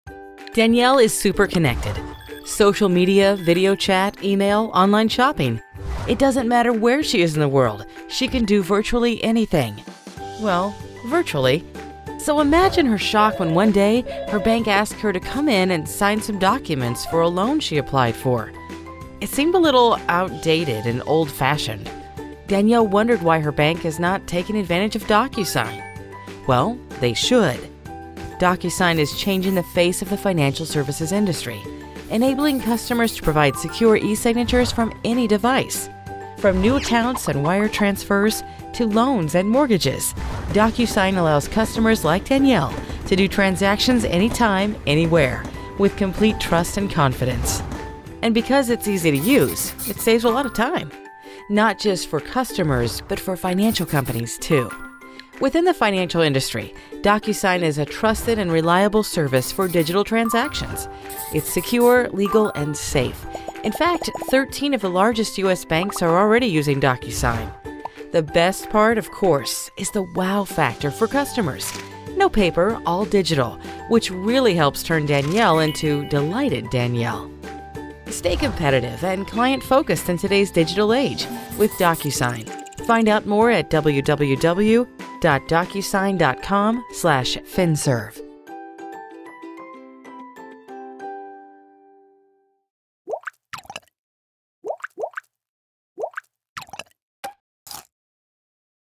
DocuSign | Explainer Video by Yum Yum Videos
we produced this animated explainer video for DocuSign, an app that is replacing printing, faxing, scanning and overnighting documents to make every approval and decision digital.